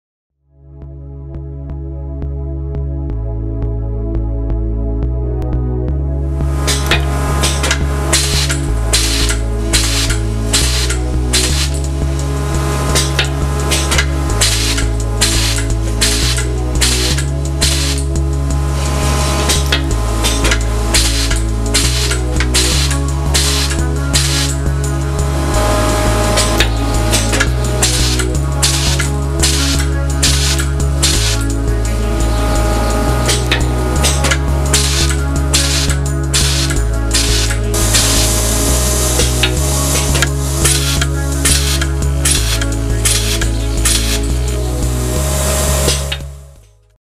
Automatic Screw Dispenser Demonstration
Watch our operator easily use a Carlson Automatic Screw Dispenser configured to release five screws into the operator's hand.